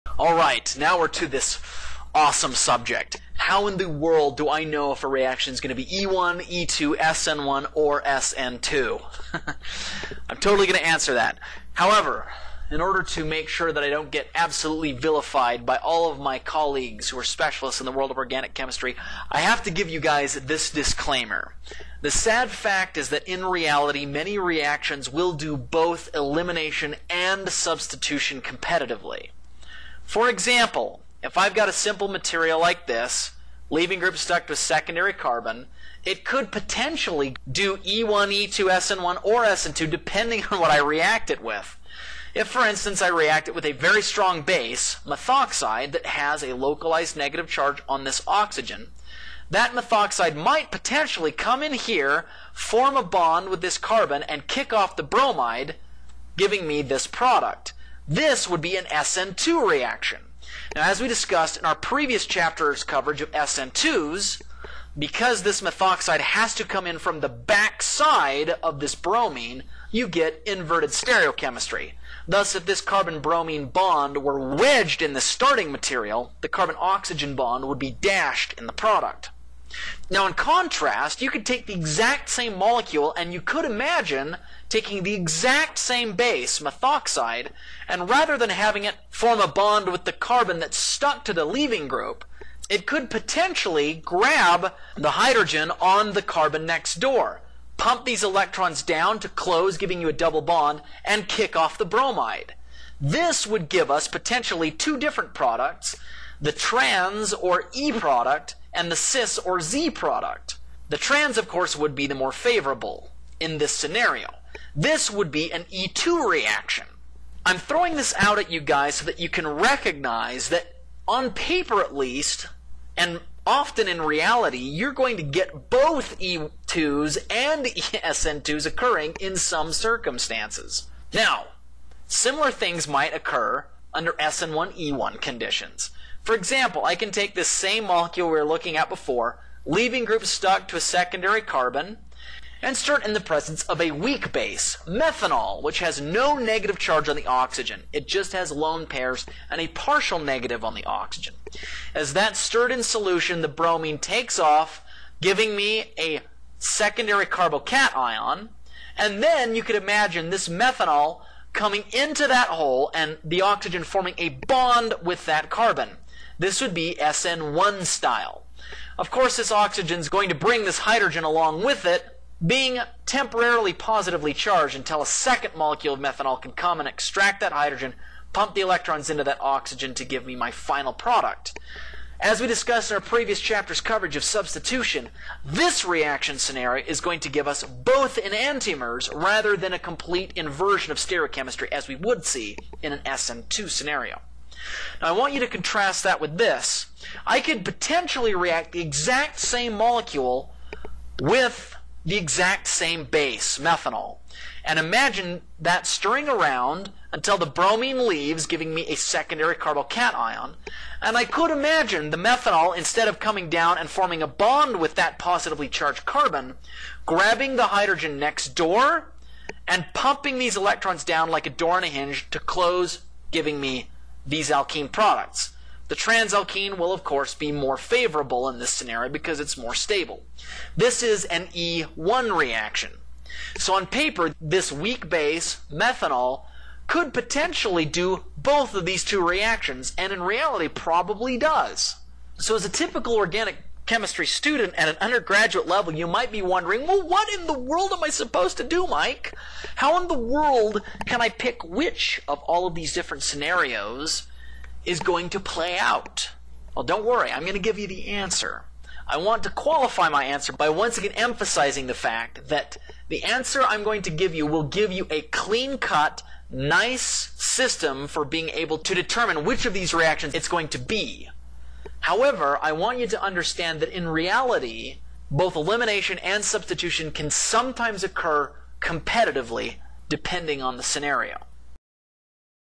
Chapter 9 – Elimination Reactions: Part 4 of 8
In this video I’ll start showing you how to predict whether a reaction will favor SN1, SN2, E1, or E2.